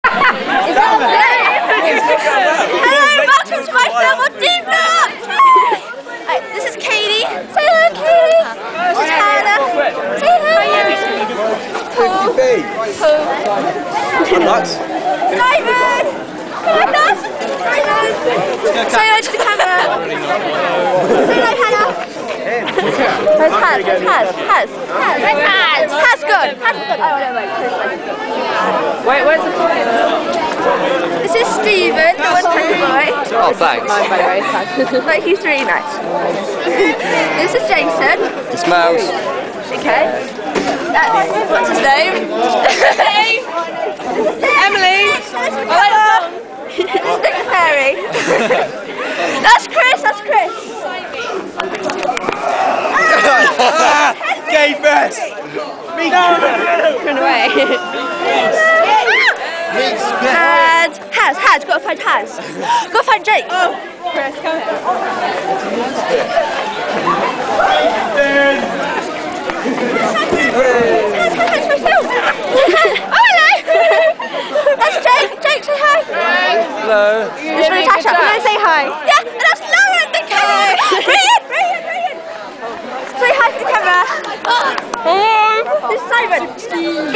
D-block Losers Ruled Yr 11 ! =]
Silly film of the yr 11 D-block losers @ Ferndown Upper School @ the end of da skl yr 2006.